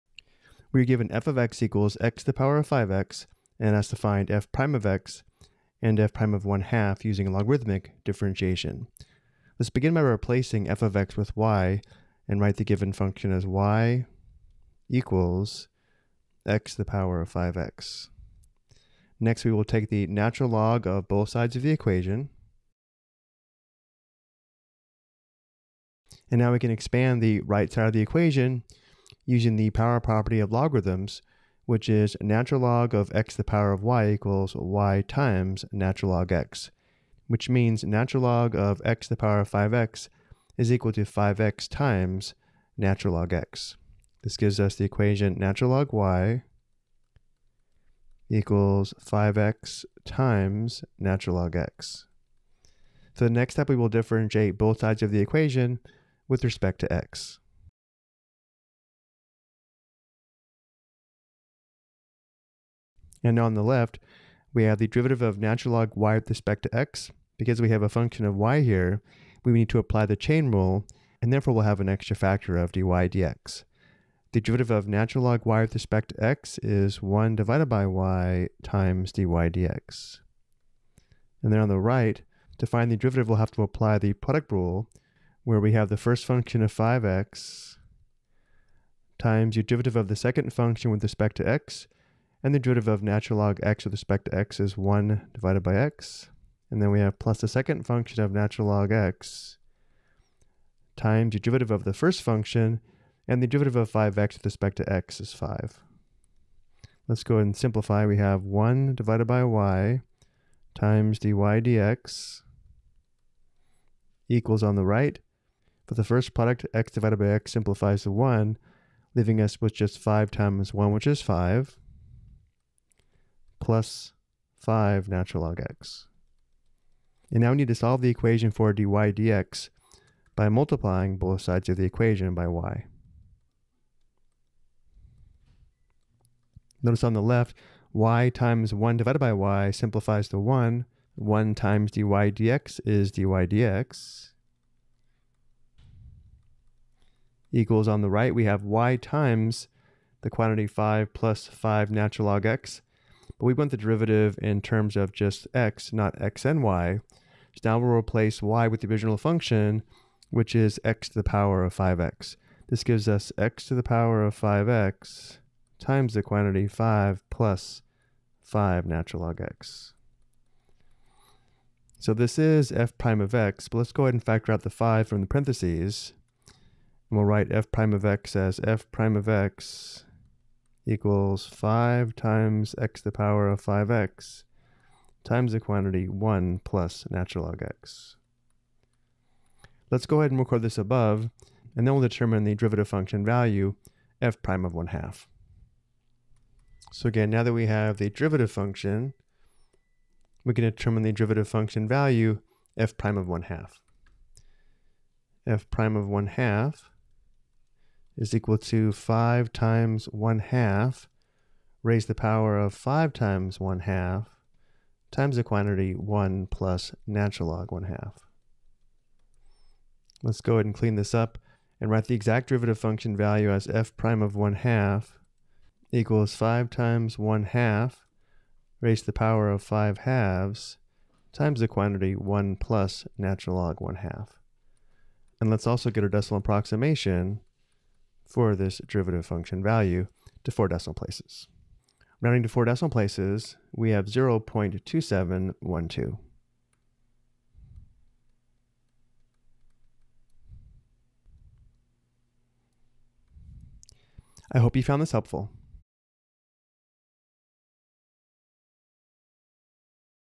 Logarithmic Differentiation: x^(ax)
This video provides an example on how to perform logarithmic differentiation.